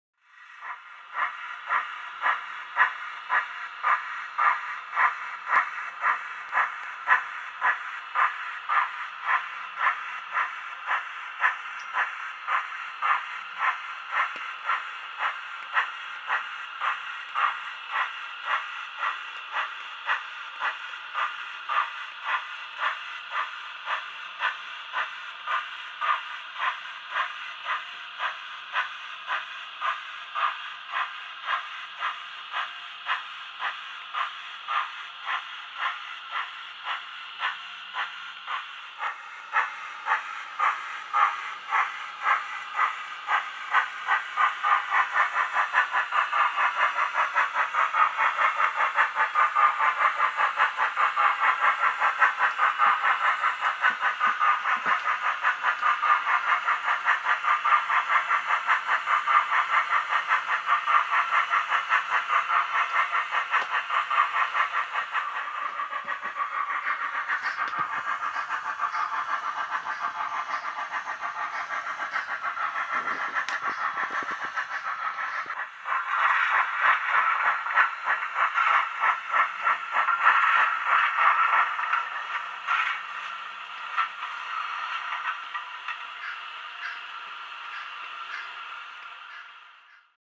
Bavarian Royal Train
This is the Roco 61472 S 3/6 of the Royal Bavarian State Railways with the private royal carriage